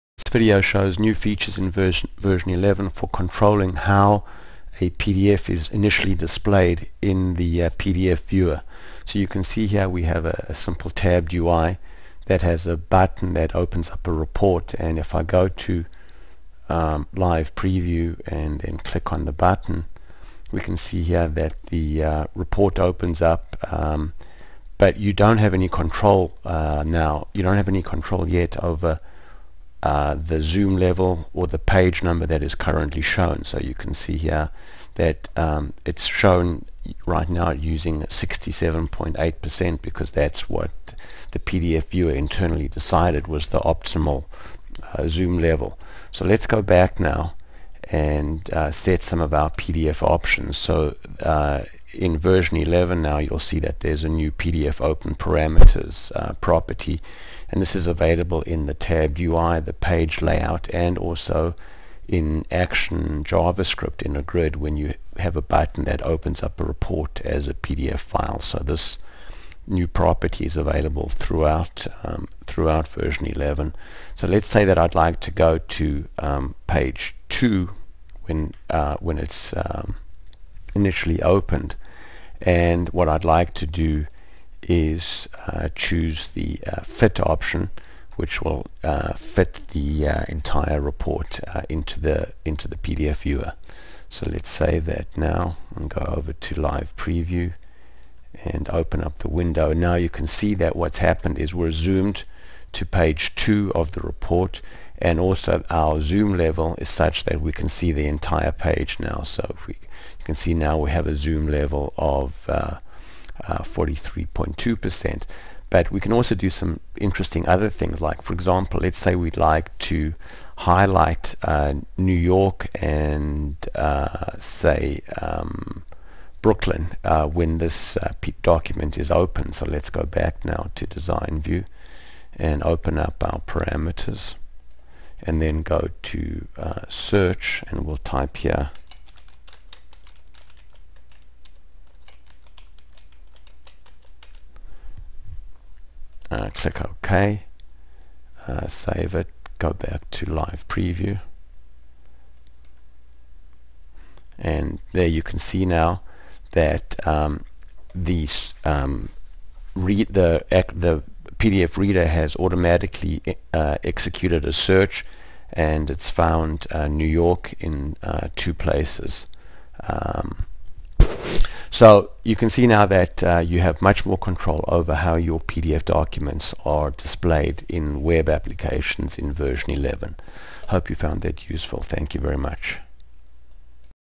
V11 Action Javascript - Opening PDF Files - New Options
M34 Alpha Five v11 PDF Open Options

 PDF Documents - Tabbed UI, Page Layout, Grid and Dialog Components - All of these components allow you to create Javascript actions that open static PDF files or dynamically generated PDF files (from Alpha Anywhere reports). Now you can control several aspects of how the PDF in initially rendered. For example, you can set the zoom level to 50% and navigate to page 2 of the document.